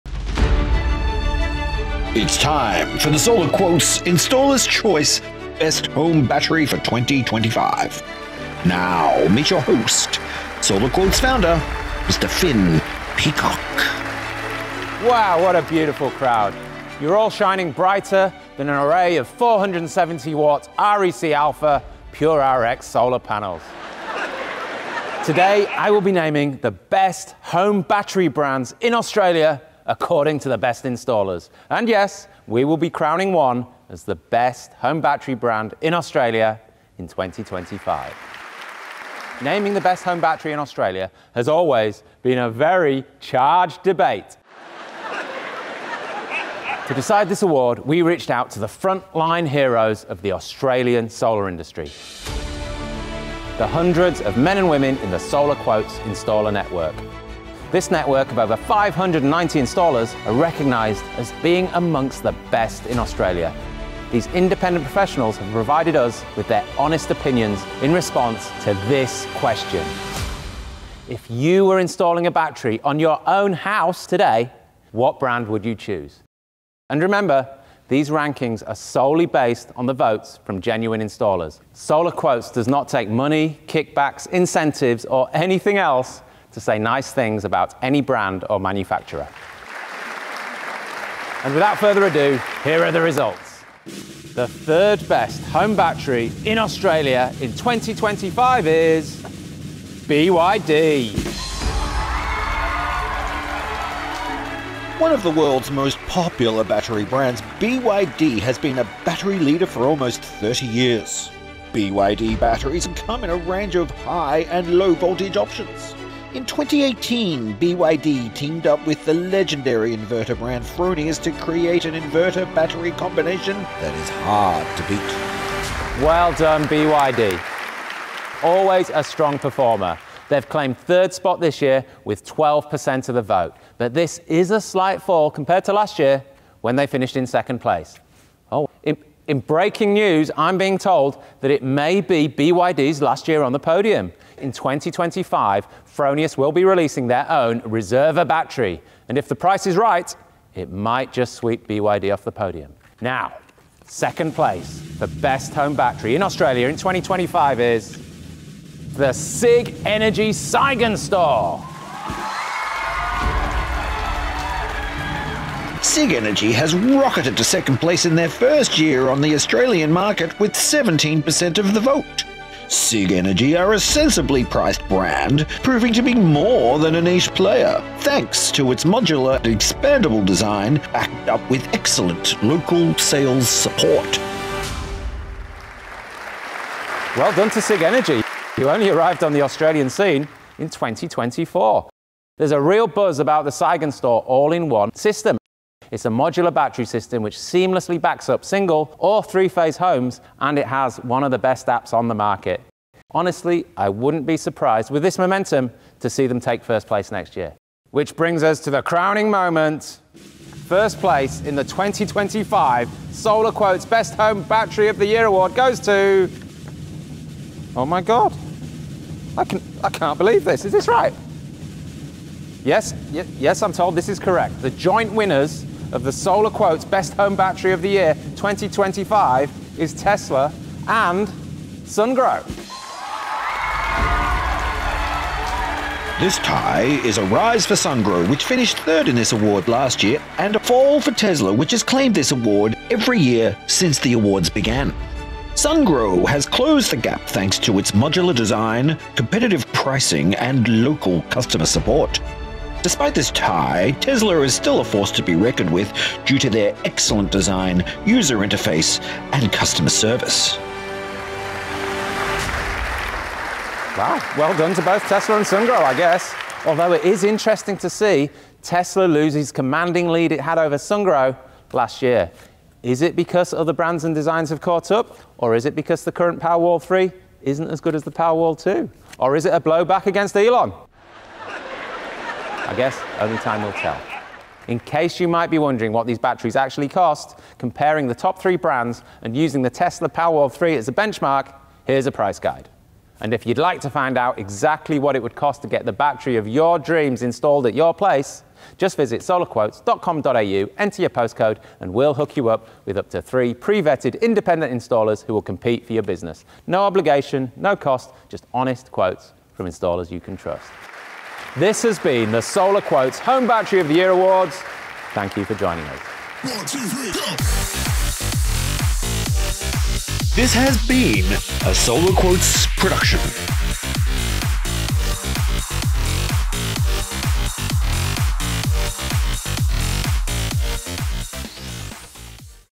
The Best Home Batteries In Australia In 2025: Installers Choice Awards
What are the best home batteries in Australia 2025? Find out how top Australian solar installers voted.

With the new federal battery rebate slashing the cost of batteries, there's been no better time than now to get quotes and explore your options.

Installers work with different brands every day - and they must support those inverters for the duration of their warranties. There's a lot riding on their choice of brand.

In the 2025 SolarQuotes Installers Choice Awards, I asked installers in the SQ network to vote for batteries they'd be happy to have on their own homes - with some surprising results.

01:40 - Third place
02:35 - Second place
03:28 - First place

*** Further info

The Federal Government Battery Rebate: What We Know (And What You Can Do Now)

Solar Batteries: Everything You Need To Know (Cost, Payback, Brands)